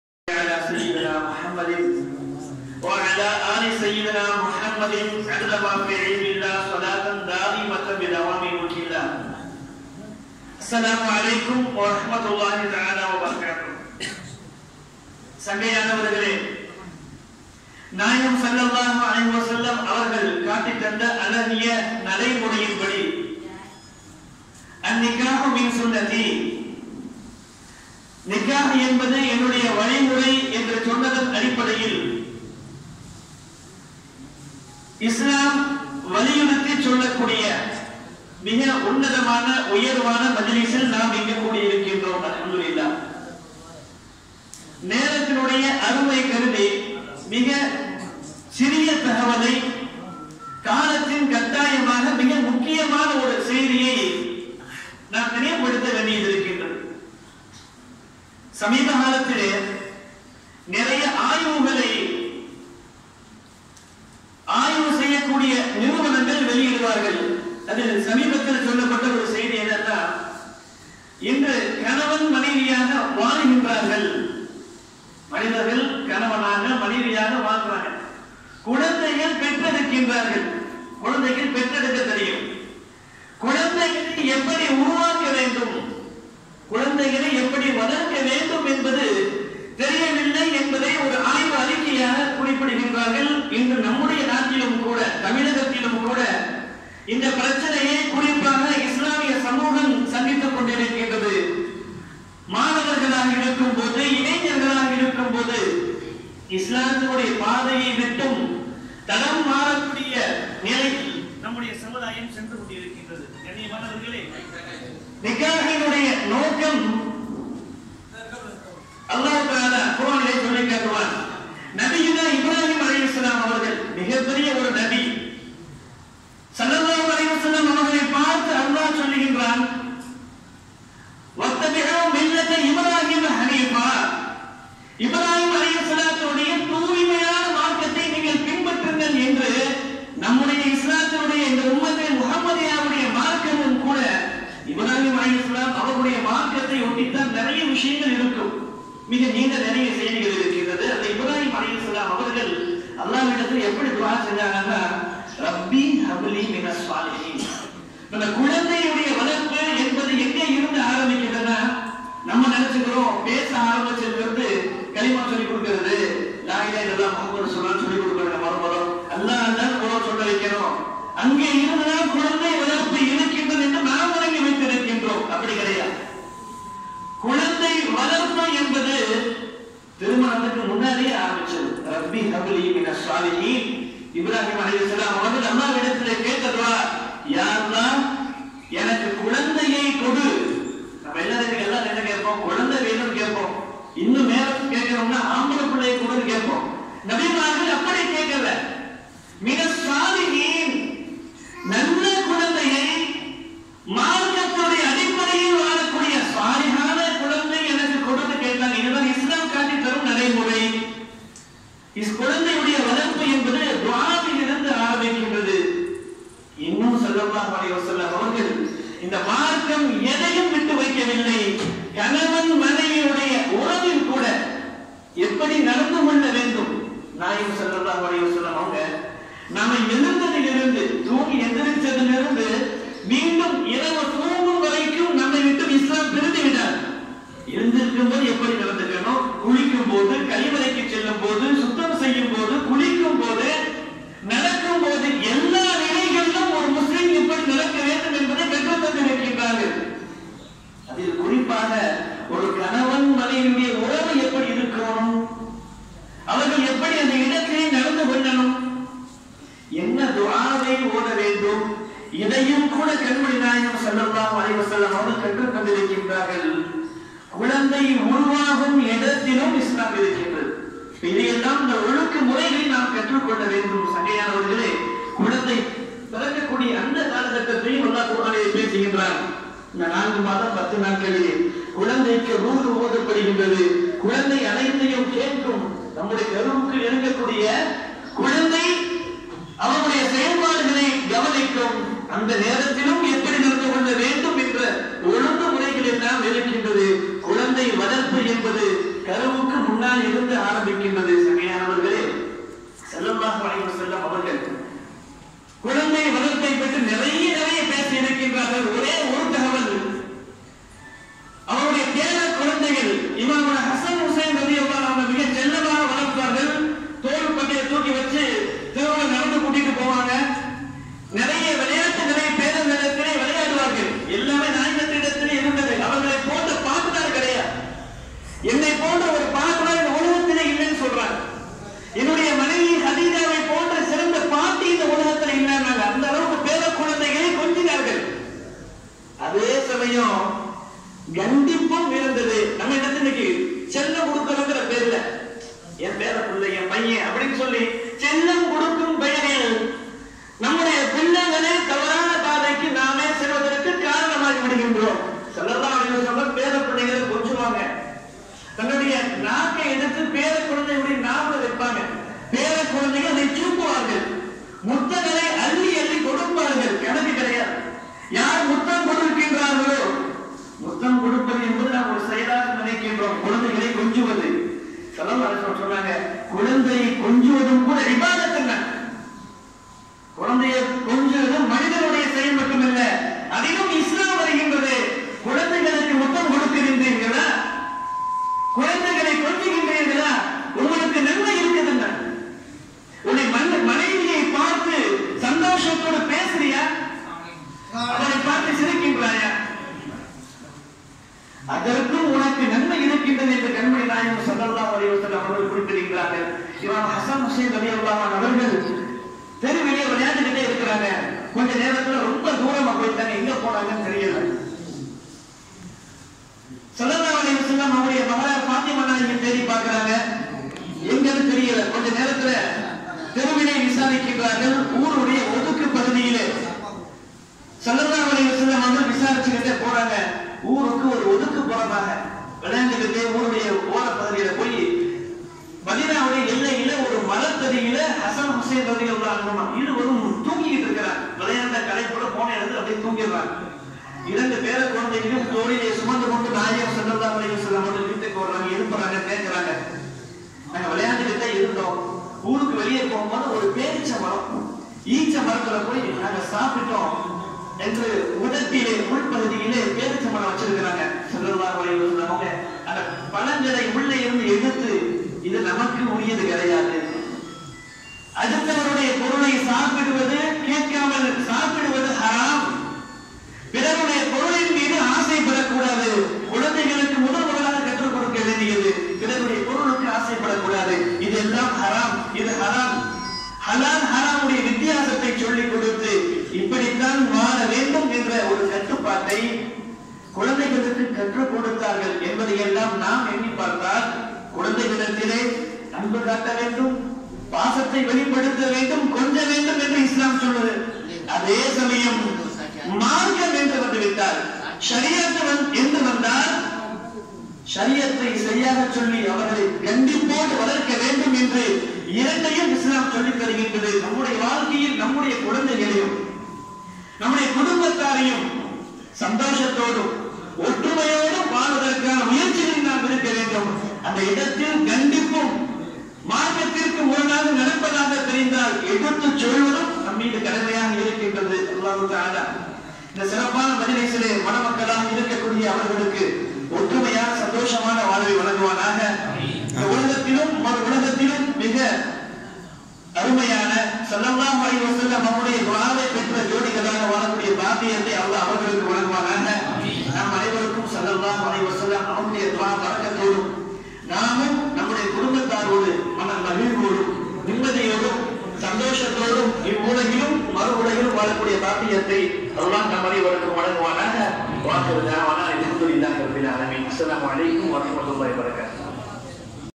நிக்காஹ் சிறப்பு பயான்.
பயான் : அல்ஹாபிழ் மௌலவி V.M.முஹம்மது ஹஸன் யாஸீனிய் ஹக்கிய்யுல் காதிரிய் அவர்கள்.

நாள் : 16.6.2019

இடம் : காரியமங்களம், மன்னார்குடி.